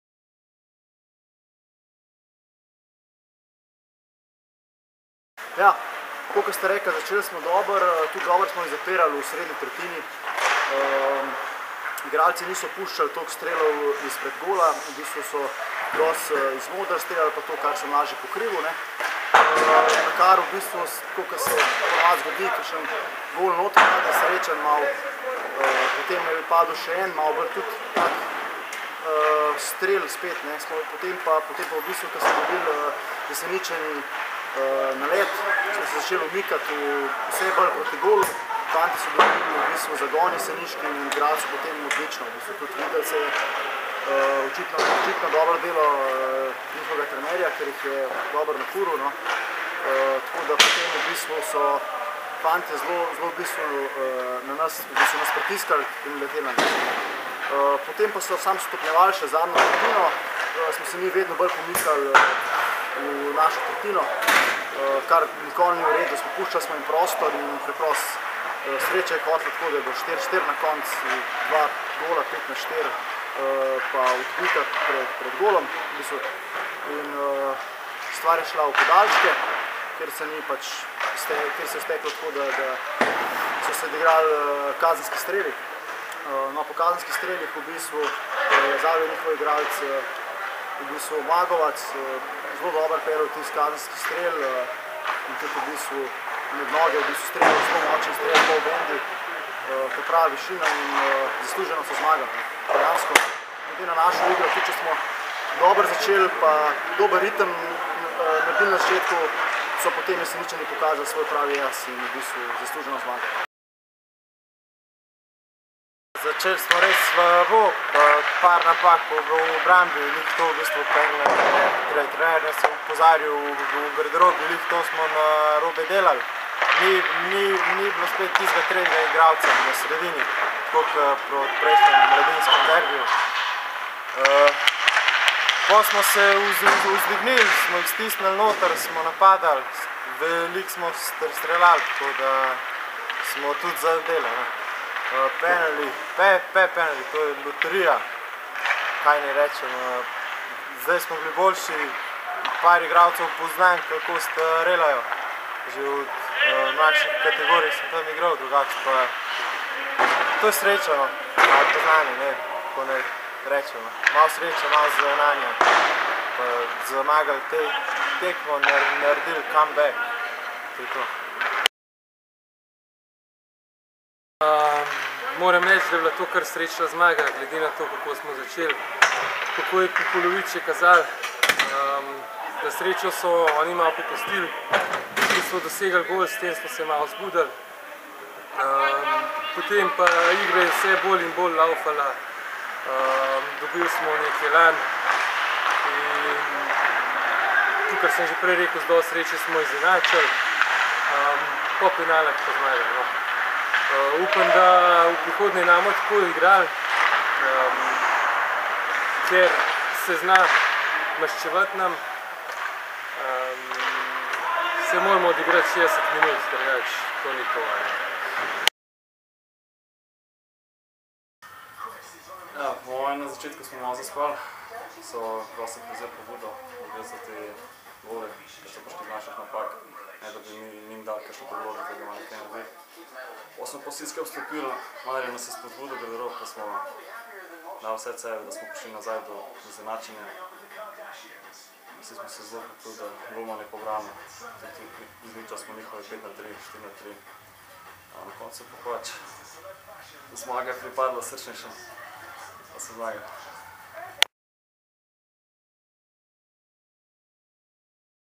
Izjave po tekmi HD mladi Jesenice : HK Olimpija
V Slohokej ligi sta podmladka Jesenic in Olimpije odigrala pravi prvenstveni derbi, z veliko streli, veliko zadetki, velikim proebratom Jesenic, na koncu pa je odločitev o zmagovalcu prineslo izvajanje kazenskih strelov. Odličen derbi zahteva nekaj izjav in vratar Matic Boh pri Olimpiji, Jure in Januš Pavlič ter izvajalec odločilnega kazenskega strela Aleksandar Magovac so povedali nekaj o tem srečanju.